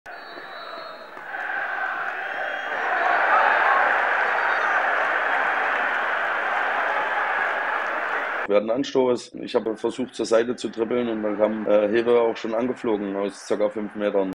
⚠️⚠️⚠️Frühes Ausrufezeichen gesetzt!😉😂
🙏🙏🙏Am Sonntag ist es nach 4,5 Jahren endlich wieder soweit: Das Thüringen-Derby zwischen dem FC Rot-Weiß Erfurt und dem FC Carl Zeiss Jena steht an!🔥🔥🔥

Wir erinnern uns daher gerne an folgende Archivperle aus dem Jahr 2006:

Im Thüringenpokalspiel Erfurt gegen Jena setzte RWE-Kicker Ronny Hebestreit mit Anpfiff ein besonderes Zeichen. Gegenspieler Torsten Ziegner erinnert sich 16 Jahre später immer noch bestens an diese Aktion. 😉😂